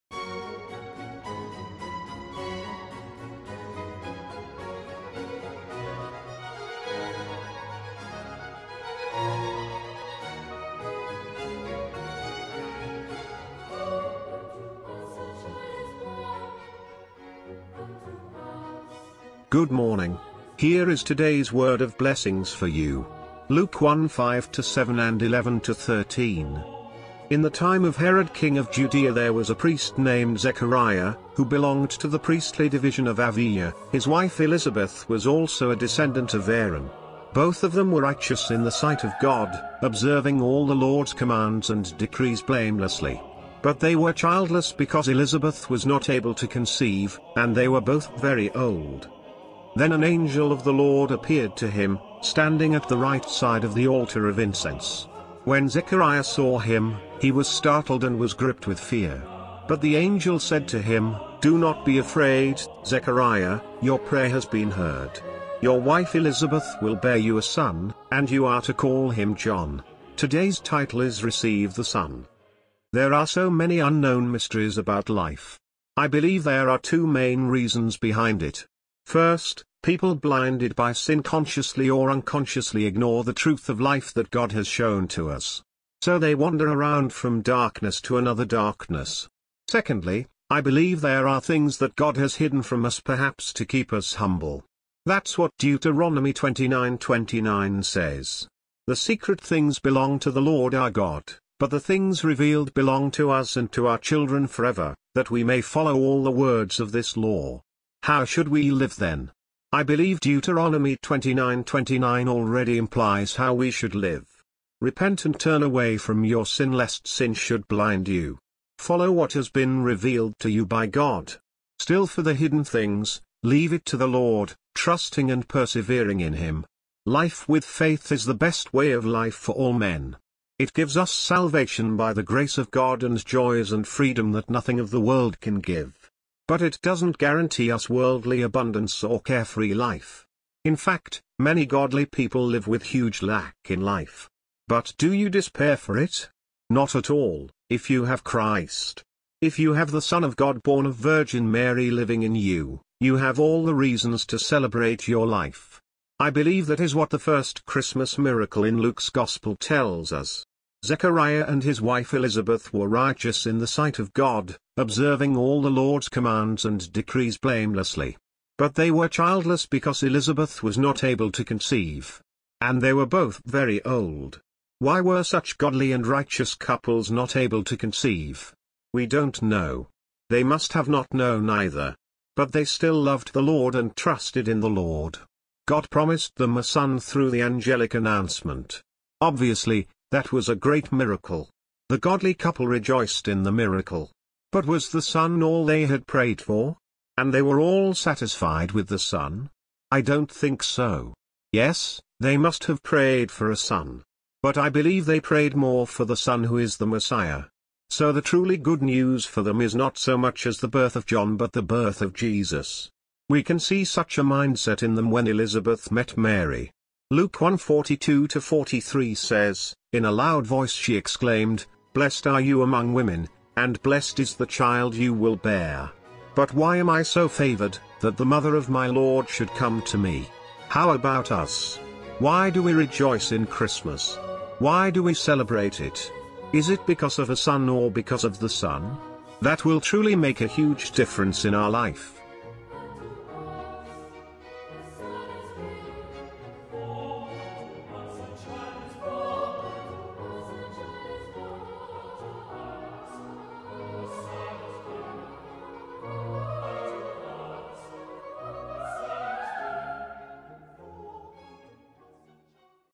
The Son Is Given (Luke 1:5-7, 11-13) Mission Blessings
But the angel said to him: “Do not be afraid, Zechariah; your prayer has been heard. Your wife Elizabeth will bear you a son, and you are to call him John.

Luke 1:5-7,  11-13

 5 In the time of Herod king of Judea there was a priest named Zechariah, who belonged to the priestly division of Abijah; his wife Elizabeth was also a descendant of Aaron. 6 Both of them were righteous in the sight of God, observing all the Lord’s commands and decrees blamelessly. 7 But they were childless because Elizabeth was not able to conceive, and they were both very old.
11 Then an angel of the Lord appeared to him, standing at the right side of the altar of incense. 12 When Zechariah saw him, he was startled and was gripped with fear. 13 But the angel said to him: “Do not be afraid, Zechariah; your prayer has been heard. Your wife Elizabeth will bear you a son, and you are to call him John.

  Today’s title is  “The Son Is Given”

         There are so many unknown mysteries about life.    I believe there are two main reasons behind it.  First, people blinded by sin consciously or unconsciously ignore the truth of life that  God has shown to us.  So they wander around from darkness to another darkness.  Secondly, I believe there are things that God has hidden from us perhaps to keep us humble.   That’s what Deuteronomy 29:29 says.  “The secret things belong to the Lord our God, but the things revealed belong to us and to our children forever, that we may follow all the words of this law.”.   How should we live then?  I believe Deuteronomy 29:29 already implies how we should live.    Repent and turn away from your sin lest sin should blind you.  Follow what has been revealed to you by God.  Still for the hidden things, leave it to the Lord, trusting and persevering in Him.   Life with faith is the best way of life for all men.    It gives us salvation by the grace of God and joys and freedom that nothing of the world can give.  But it doesn’t guarantee us worldly abundance or carefree life.  In fact, many godly people live with huge lack in life.  But do you despair for it?  Not at all, if you have Christ.   If you have the Son of God born of virgin Mary living in you, you have all the reasons to celebrate your life.  I believe that is what the first Christmas miracle in Luke’s gospel tells us.   Zechariah and his wife Elizabeth were righteous in the sight of God, observing all the Lord’s commands and decrees blamelessly. But they were childless because Elizabeth was not able to conceive. and they were both very old.   Why were such godly and righteous couples not able to conceive?   We don’t know.   They must have not known either.  But they still loved the Lord and trusted in the Lord.  God promised them a son through the angelic announcement.   Obviously, that was a great miracle.   The godly couple rejoiced in the miracle.  But was the son all they had prayed for?  And they were all satisfied with the son?   I don’t think so.   Yes, they must have prayed for a son.  But I believe they prayed more for “The Son” who is the messiah.   So the truly good news for them is not so much as the birth of John but the birth of Jesus.   We can see such a mindset in them when Elizabeth met Mary.  Luke 1:42-43 says, “ In a loud voice she exclaimed: “Blessed are you among women, and blessed is the child you will bear!  But why am I so favored, that the mother of my Lord should come to me?”   How about us?   Why do we rejoice in Christmas?  Why do we celebrate it?   Is it because of "a" son or because of "the" Son?   That will truly make a huge difference in our life.